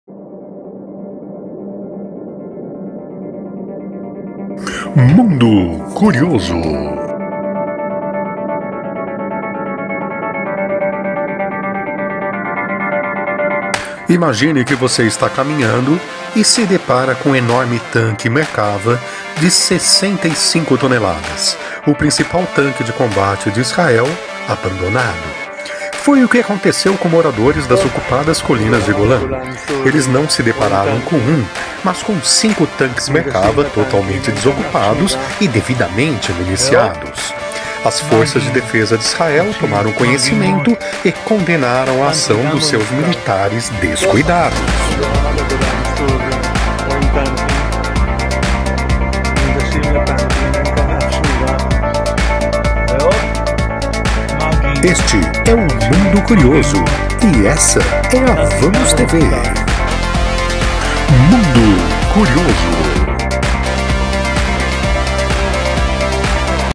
MUNDO CURIOSO - Colinas de Golan: tanques ativos gigantescos, repletos de armamentos, abandonados
MUNDO CURIOSO

Colinas de Golan – Área Síria ocupada por Israel.

Imagine que você está caminhando e se depara com um enorme tanque Merkava de 65 toneladas, o principal tanque de combate de Israel, abandonado.

Foi o que aconteceu com moradores das ocupadas Colinas de Golan. Eles não se depararam com um, mas com 5 Tanques Merkava totalmente desocupados e devidamente municiados.

As Forças de Defesa de Israel tomaram conhecimento e condenaram a ação dos seus militares descuidados.

Imagens: Pixabay, Wikipedia e @Ofer_Hadad

VAMOS TV, uma nova forma de fazer canal no YouTube!